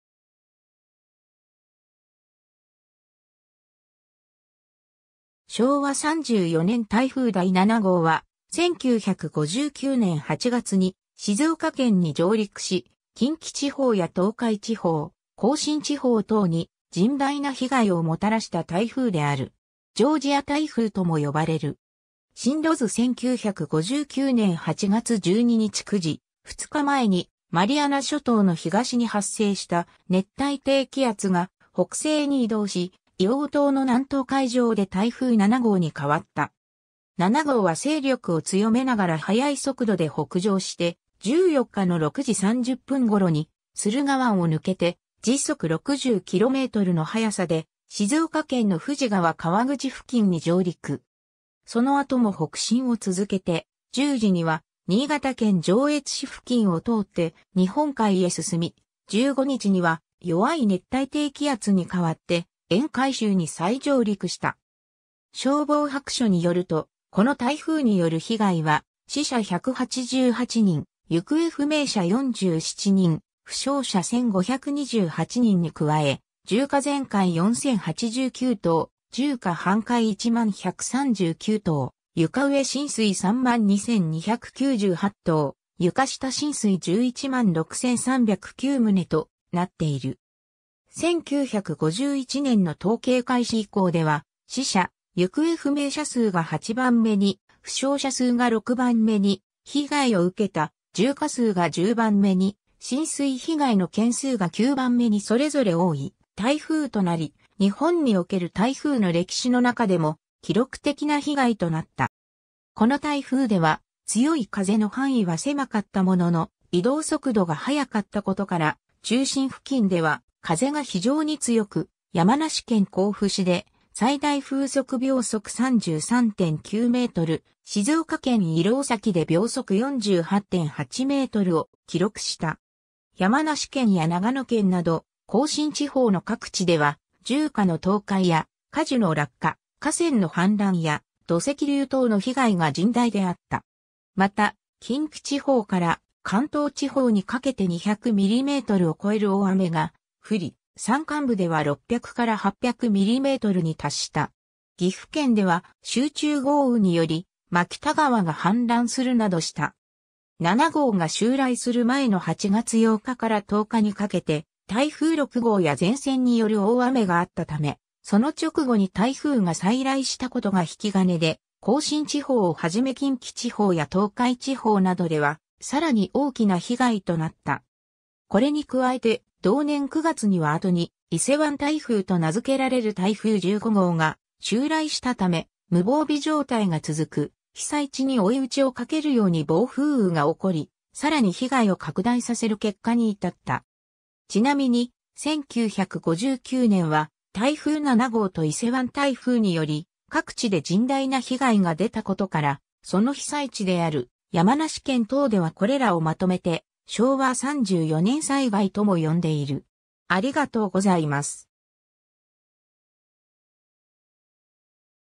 昭和34年台風第7号
昭和34年台風第7号（しょうわ34ねんたいふうだい7ごう、国際名：ジョージア / Georgia）は、1959年8月に静岡県に上陸し、近畿地方や東海地方、甲信地方等に甚大な被害をもたらした台風である。
ジョージア台風とも呼ばれる。
 進路図 1959年8月12日9時、2日前にマリアナ諸島の東に発生した熱帯低気圧が北西に移動し、硫黄島の南東海上で台風7号に変わった。
7号は勢力を強めながら速い速度で北上して、14日の6時30分頃に、駿河湾を抜けて時速60 kmの速さで静岡県の富士川河口付近に上陸。
その後も北進を続けて、10時には新潟県上越市付近を通って日本海へ進み、15日には弱い熱帯低気圧に変わって沿海州に再上陸した。
 消防白書によると、この台風による被害は、死者188人・行方不明者47人・負傷者1,528人に加え、住家全壊4,089棟・住家半壊10,139棟・床上浸水32,298棟・床下浸水116,309棟となっている。
1951年の統計開始以降では、死者・行方不明者数が8番目に、負傷者数が6番目に、被害を受けた住家数が10番目に、浸水被害の件数が9番目にそれぞれ多い台風となり、日本における台風の歴史の中でも記録的な被害となった。
 この台風では、強い風の範囲は狭かったものの、移動速度が速かったことから中心付近では風が非常に強く、山梨県甲府市で最大風速33.9 m/s（最大瞬間風速43.2 m/s）、静岡県石廊崎で48.8 m/s（同64.0 m/s）を記録した。
 山梨県や長野県など甲信地方の各地では、住家の倒壊や果樹の落下、河川の氾濫や土石流等の被害が甚大であった。
 また、近畿地方から関東地方にかけて200 mmを超える大雨が降り、山間部では600～800 mmに達した。
岐阜県では、集中豪雨により牧田川が氾濫するなどした。
7号が襲来する前の8月8日から10日にかけて、台風6号や前線による大雨があったため、その直後に台風が再来したことが引き金で、甲信地方をはじめ近畿地方や東海地方などではさらに大きな被害となった。
これに加えて、同年9月には後に伊勢湾台風と名付けられる台風15号が襲来したため、無防備状態が続く被災地に追い討ちをかけるように暴風雨が起こり、さらに被害を拡大させる結果に至った。
ちなみに、1959年は台風7号と伊勢湾台風により各地で甚大な被害が出たことから、その被災地である山梨県等ではこれらをまとめて「昭和34年災害」とも呼んでいる。